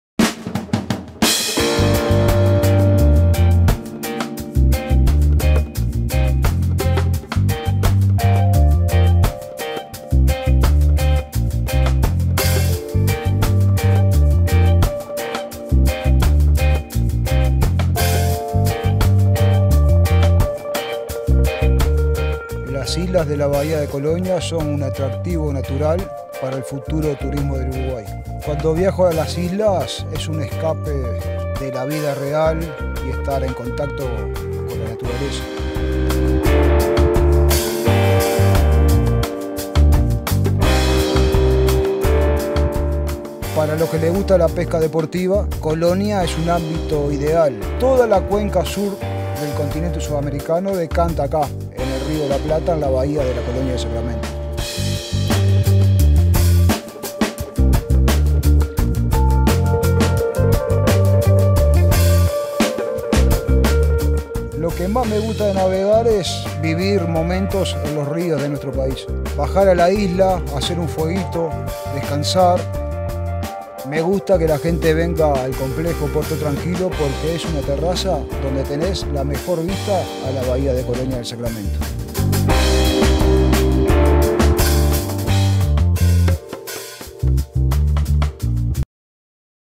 LA COLONIA DEL SACRAMENTO Y SUS ISLAS
Visibles desde la costa pero escasamente exploradas, las islas que rodean la bahía de la ciudad de Colonia son un llamado a la aventura.